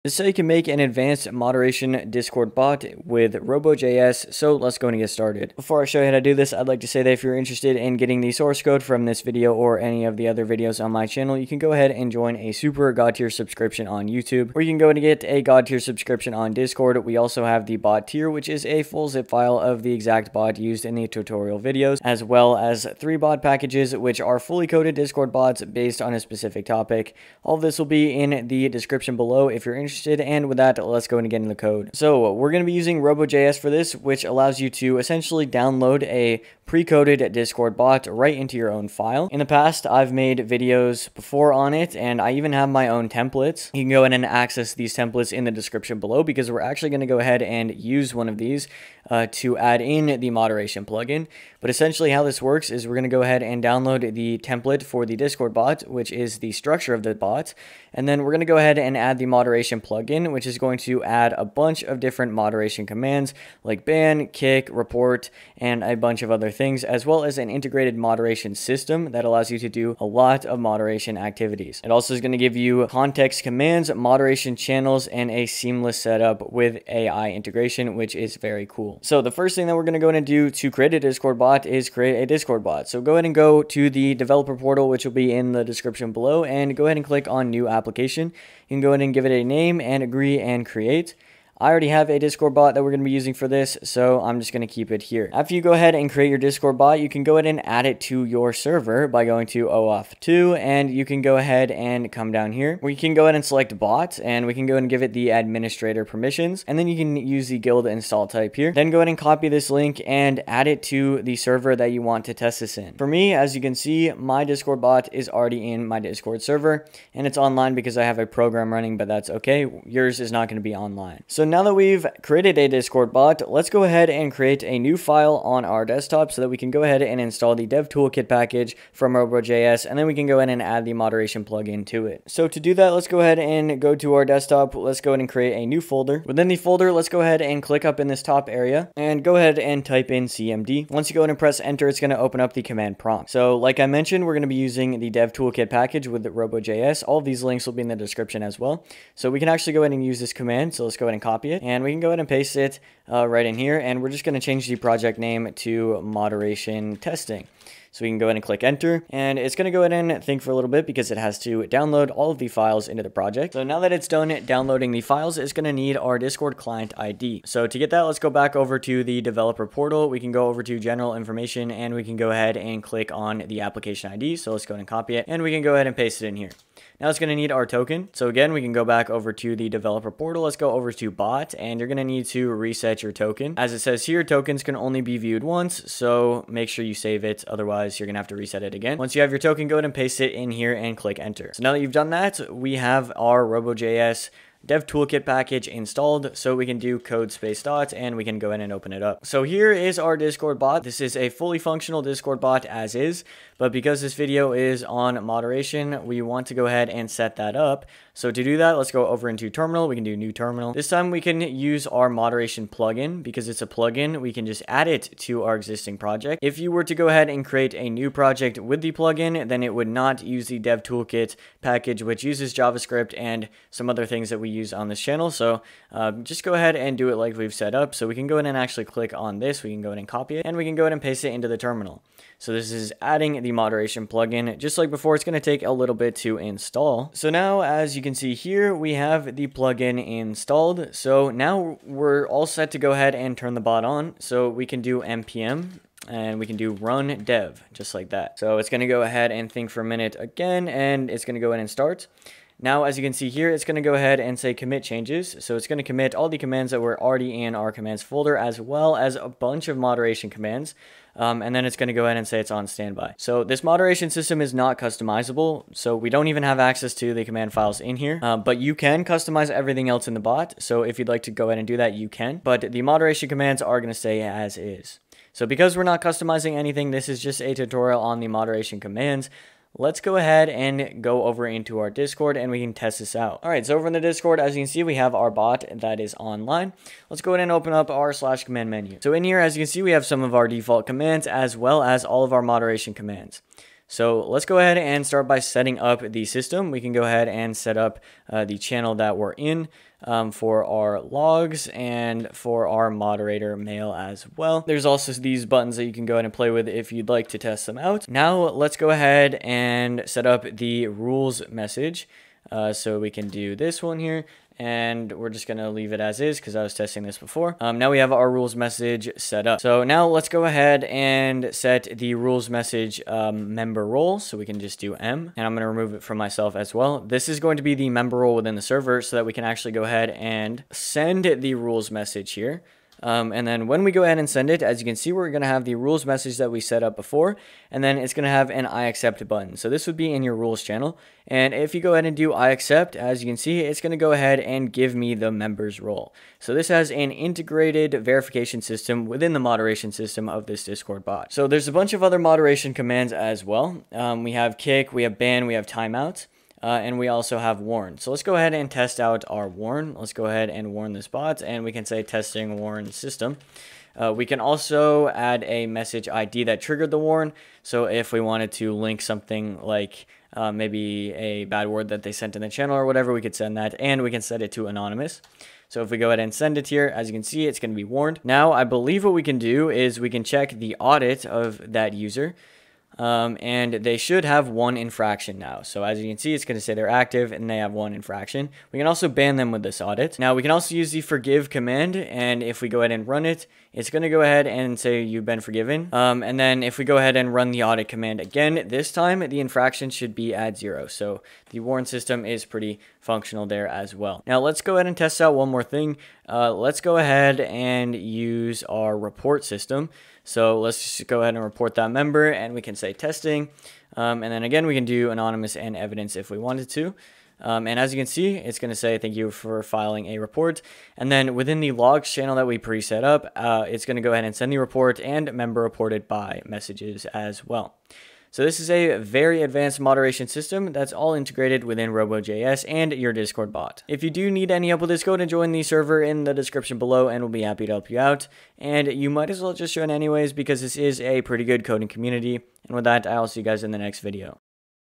[NEW] - How to make an advanced MODERATION SYSTEM for your Discord Bot! || RoboJS
This is how you can make an advanced moderation system using robojs and discord.js!